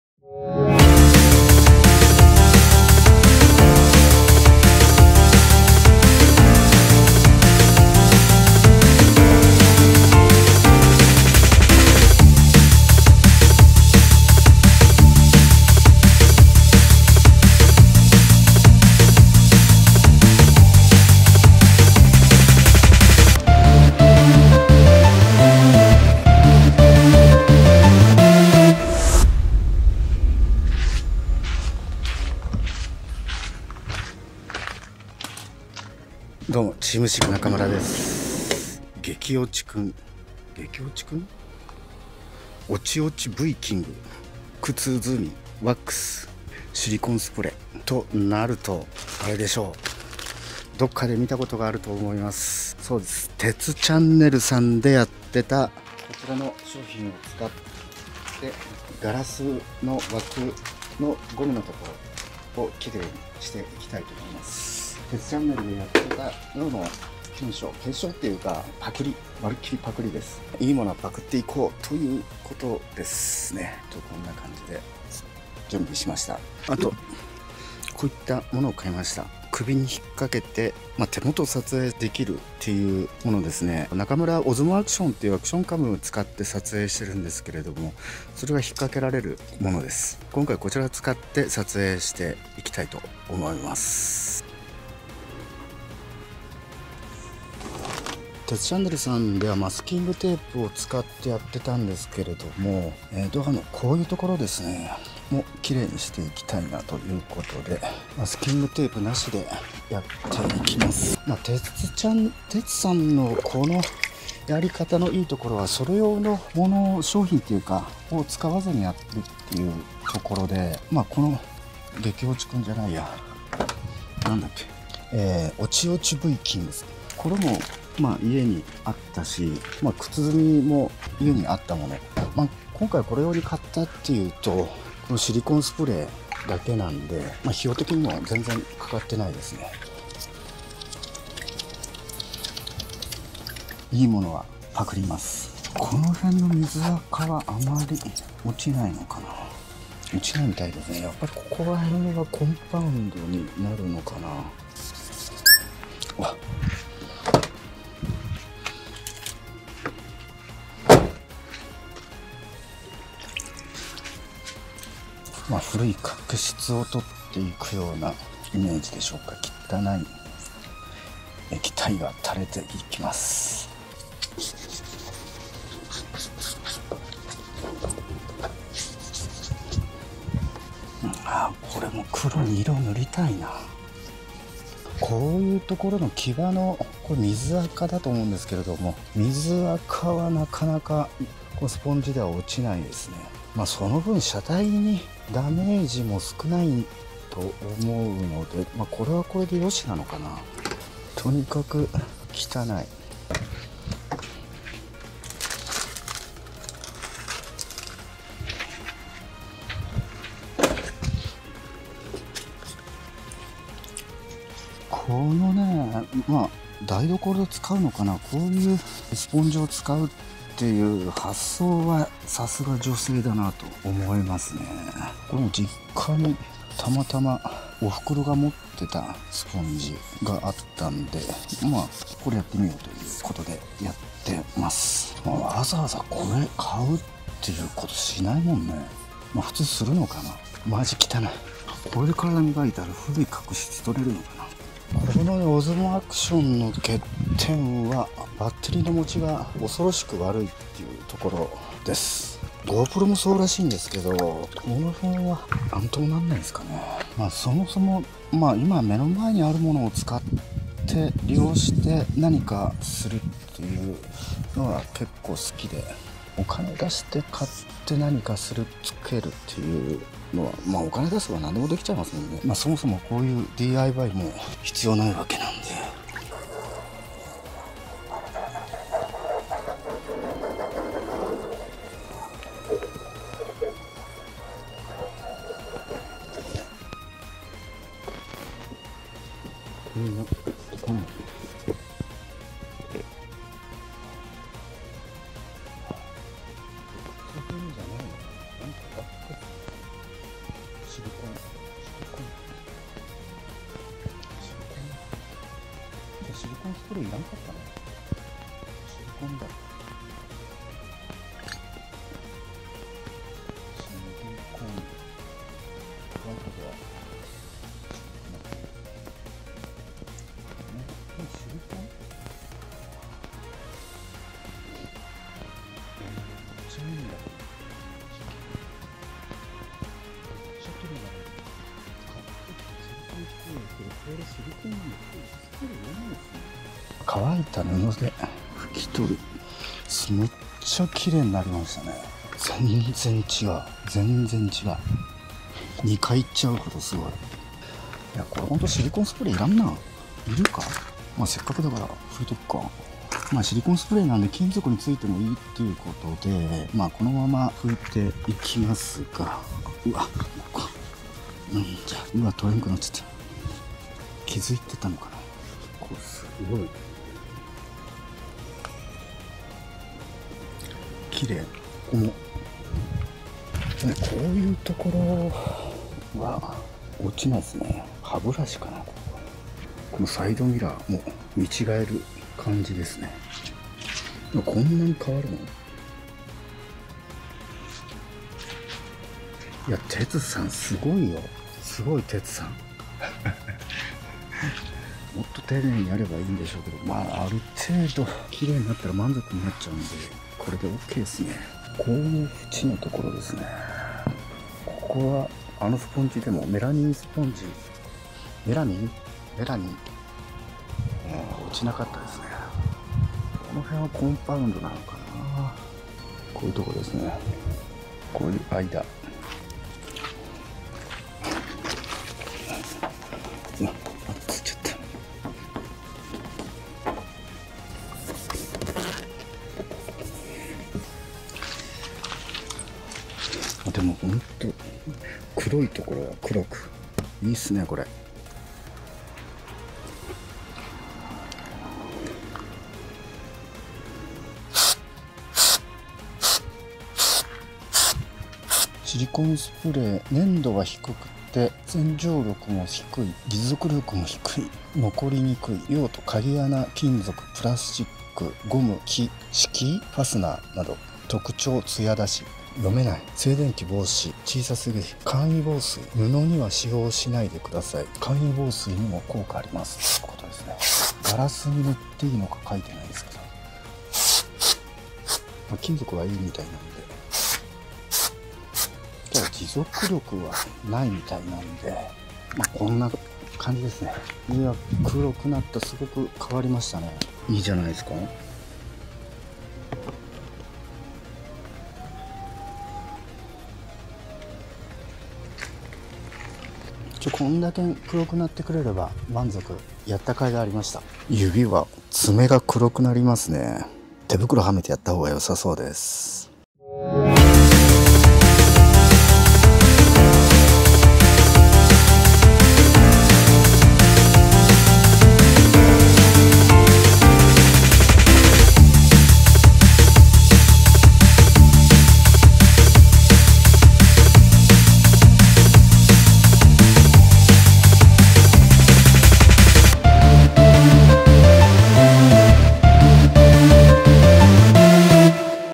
窓のゴムモールの紫外線劣化を100円グッズでピカピカ艶出しコーティング | TeamSEEKなかむら
時間の経過とともに白く劣化していったドア窓のゴムモールを、メラミンスポンジ、ワックス、シリコンスプレーで磨いてコーティングしていきます。
100均グッズでの施工です。
用意したワックスはもともとシリコンだったため、シリコンスプレーは不要だったかもしれません。それでもスプレーのおかげで艶は出ました。

靴墨だと思ってたワックスは、昔、野球のグローブに塗ってたスポーツワックスでした。

動画内でも告白してますように、今回の施工方法はtetu channelさんでやられていたもののパクリです。
それ用の材料ではなく、身近にあるものを使ってのアイディアにのっかったカタチです。
tetu channelさんありがとうございます。

■ アマゾンリンク ■
↓インバーター2000W

↓ ソーラーチャージコントローラー

↓ディープサイクルバッテリー